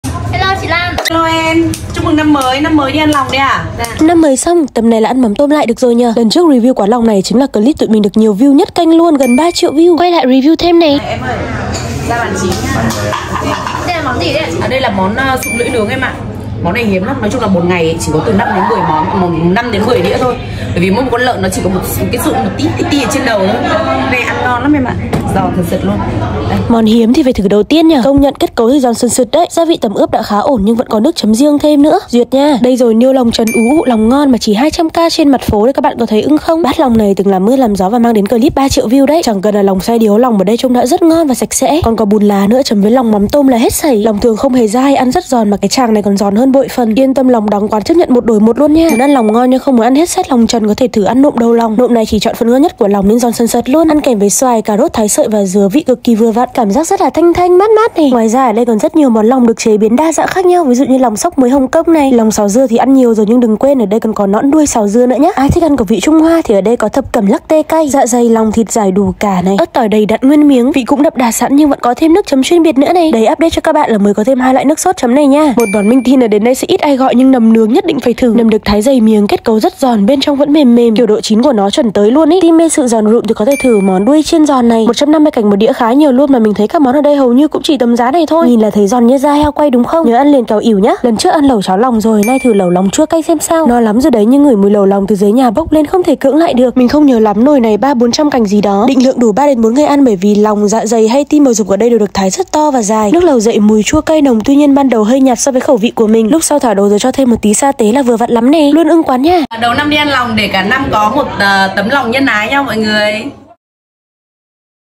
Lòng Bà Lương Siêu Ngon Phố Phan Chu Trinh| Ở Hà Nội
Quán lòng bà Lương triệu view tụi mình đã quay lại không biết bao nhiêu lần rồi. Giới thiệu bạn bè, người thân, gia đình, từ người lớn khó tính tới tụi nhỏ ở nhà ai cũng khen hết.
*Theo dõi Chúng mình tại:
© Copyright by OHANOI Channel ☞ Do not Reup